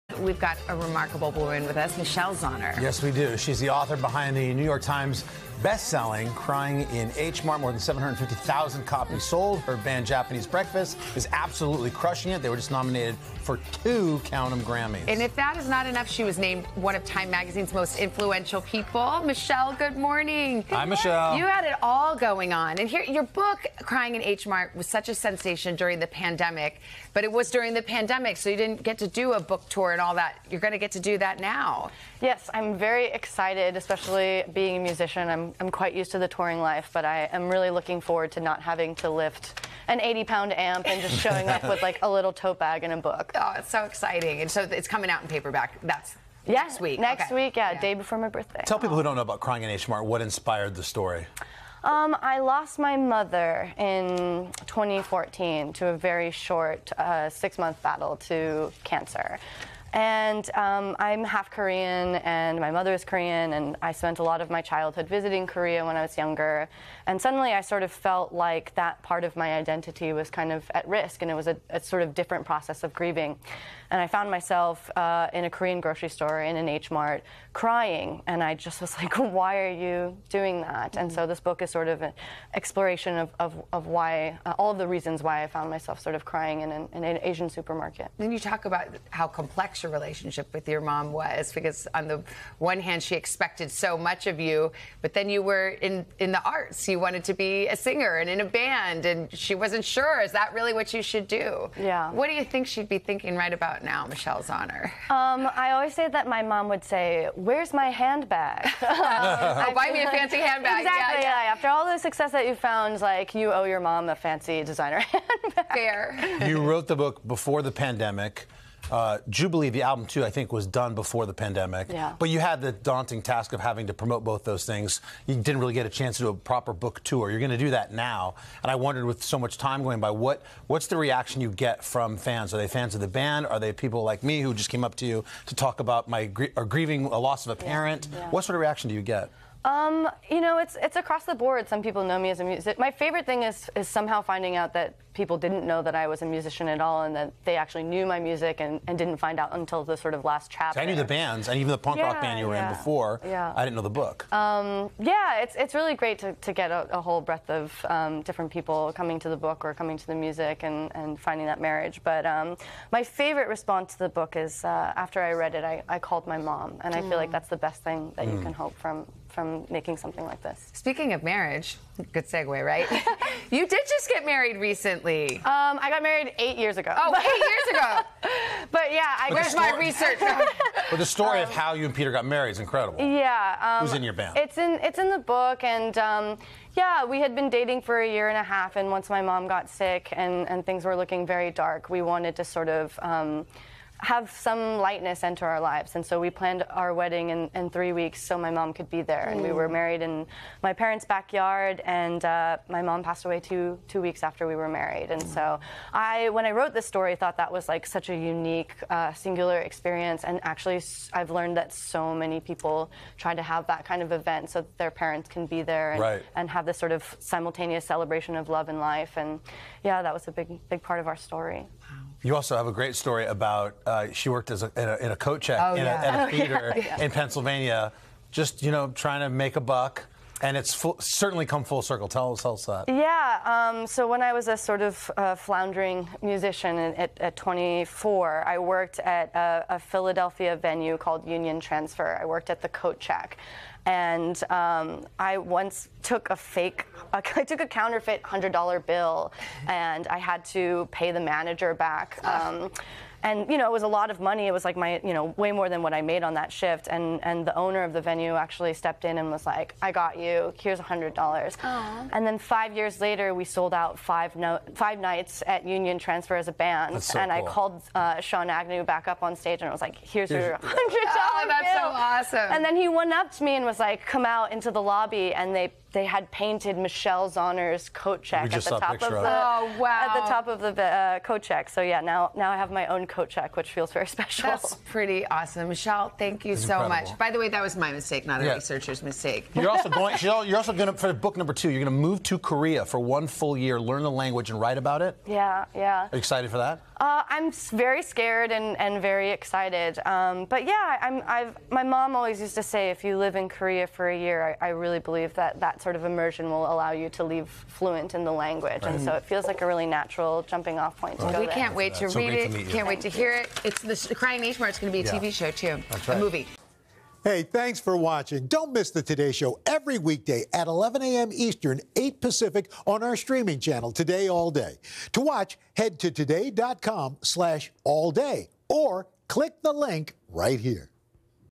Michelle Zauner on inspiration behind bestseller ‘Crying in H Mart'
Musician and author Michelle Zauner opens up about the inspiration behind her bestseller "Crying in H Mart," her band Japanese Breakfast, and what it's like having fans of the book discover her music and vice versa.